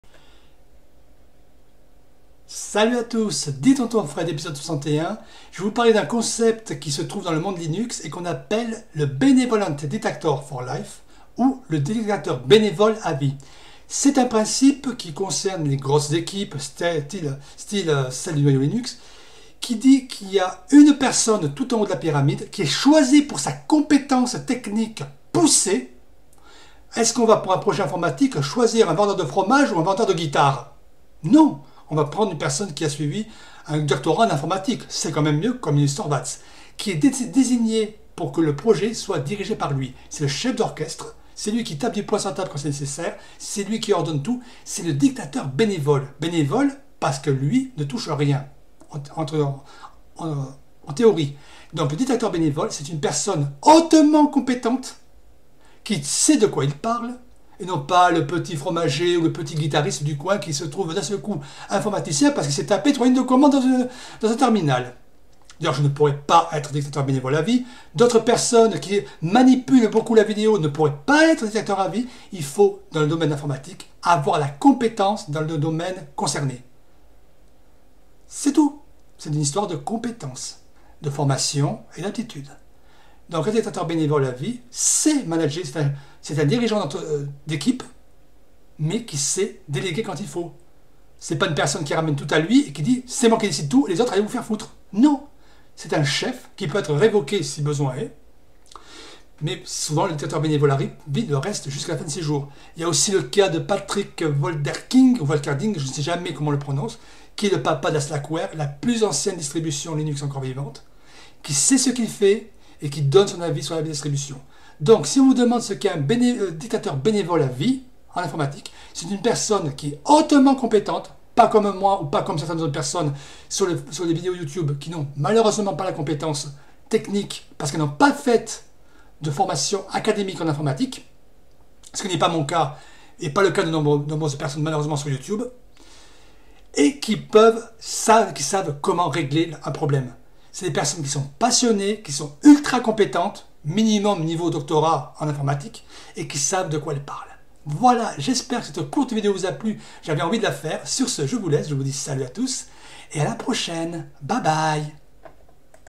Dis Tonton Fred, épisode 61 : le dictateur bénévole à vie en informatique
Et non l'épisode 60 comme je l'ai dit en introduction...